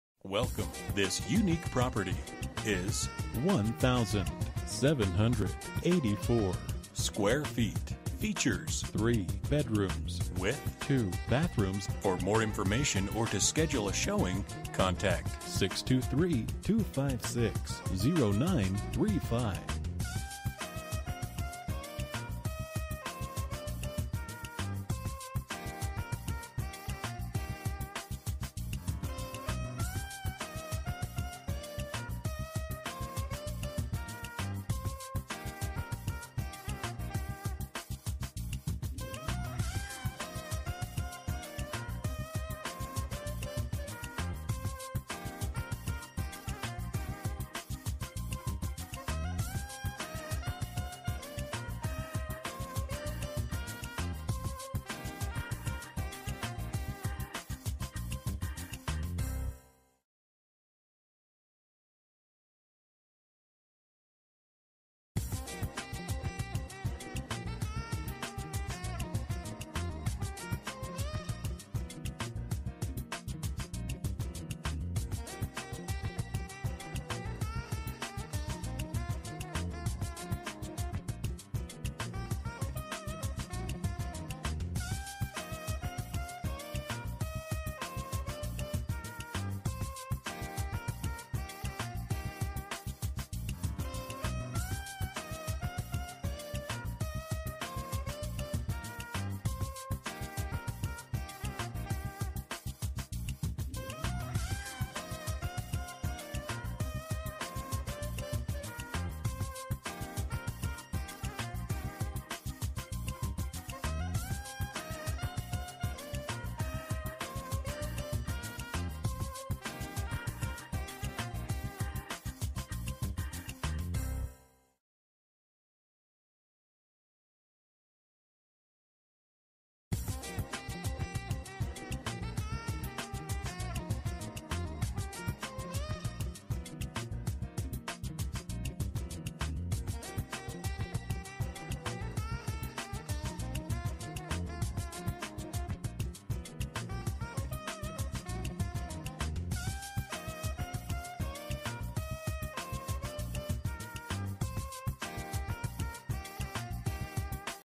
Residential for sale - 1608 N 73RD Avenue, Phoenix, AZ 85035
THIS IS THE ONE!! Completely remodeled 3 bedroom, 2 bathroom with a HUGE bonus room. New A/C unit, new kitchen cabinets and granite counter tops. New stainless steel appliances. New flooring throughout the home. Both bathrooms have been updated, new windows and doors. This beauty isn't going to last long on the market, hurry and take a look for yourself!

Bedrooms: 3
Bathrooms: 2
Square Feet: 1784
Price: $209,000

For more information about this property, please contact Santos Estrada at or . You can also text 5073372 to 67299.